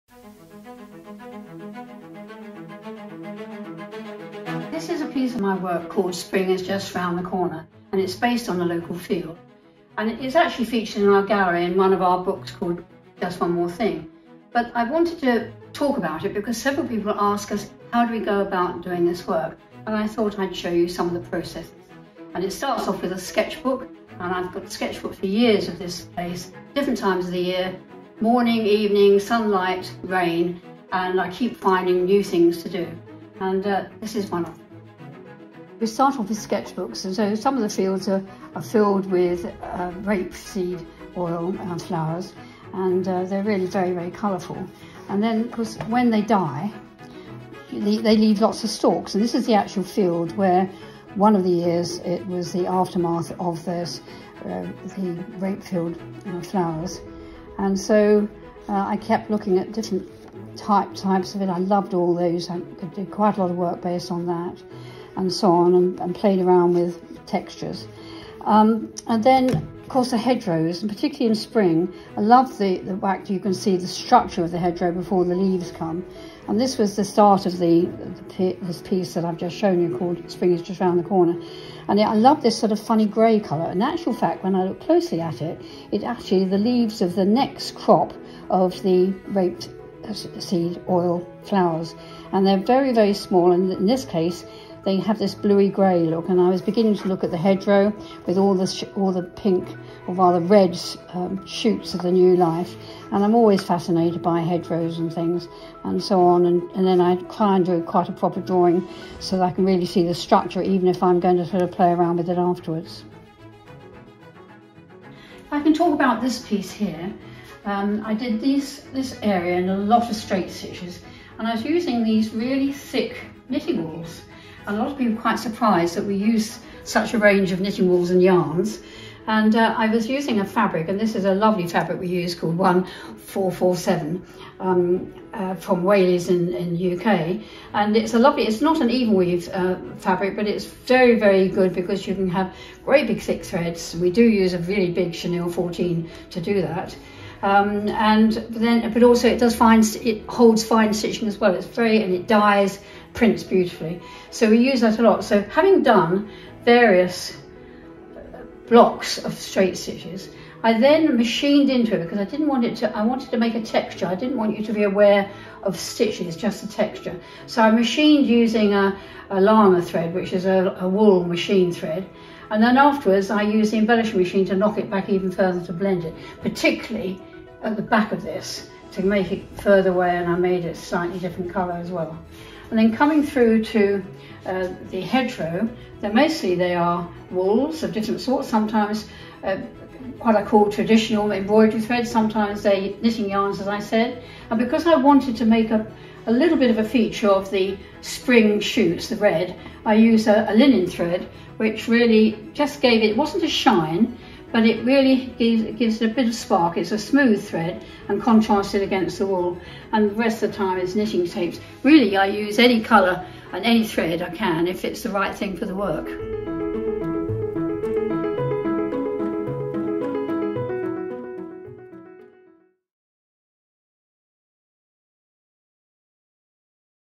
In the Studio with Jan Beaney: Spring is Just Around the Corner (Stitch & Machine Embroidery)
An Inspirational Look into the textile studio of Jan Beaney, on of the UK's pre-eminent stitch/embroidery artists living and teaching today.